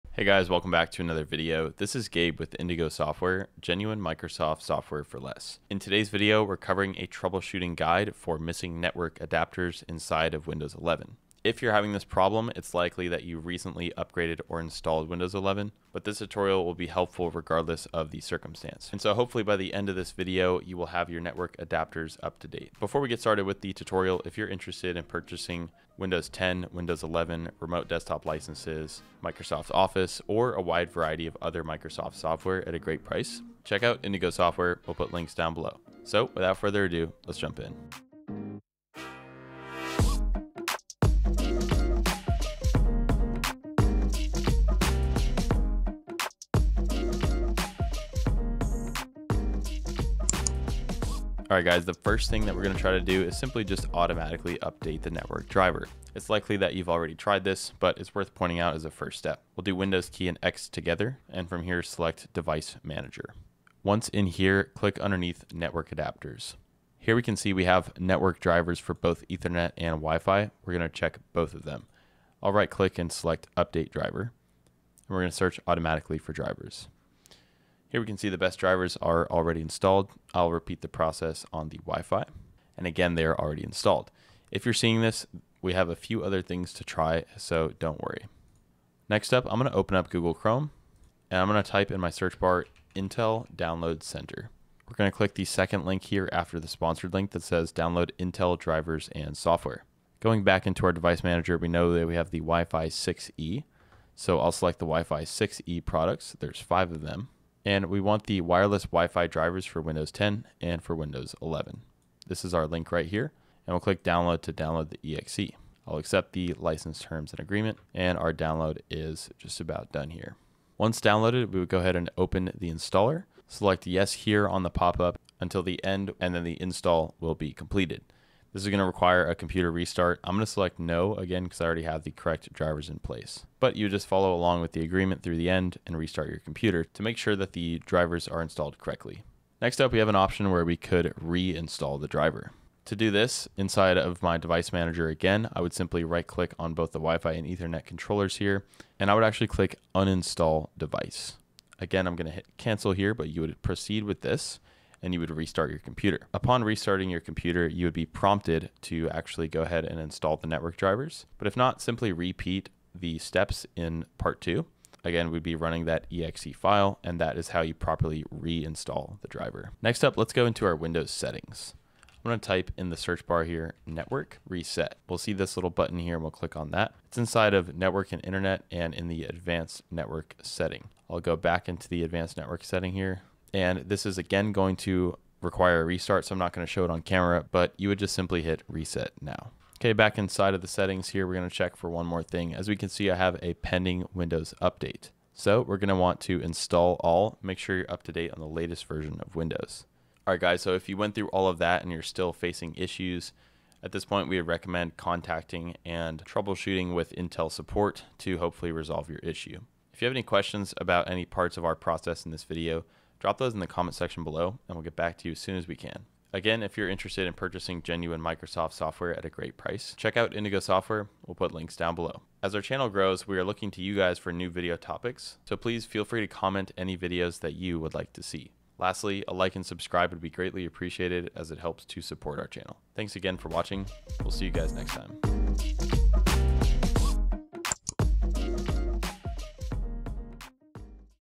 [FIXED] Intel Network Drivers Missing After Windows 11 Install/Upgrade
Are your Intel network drivers missing after upgrading or installing Windows 11? Don’t worry, I’ve got you covered! In this video, I’ll walk you through the step-by-step process to restore your Intel network drivers and get your internet connection back up and running. Whether it's a fresh installation or an upgrade, you’ll learn how to reinstall missing network drivers, troubleshoot issues, and avoid future connectivity problems on Windows 11.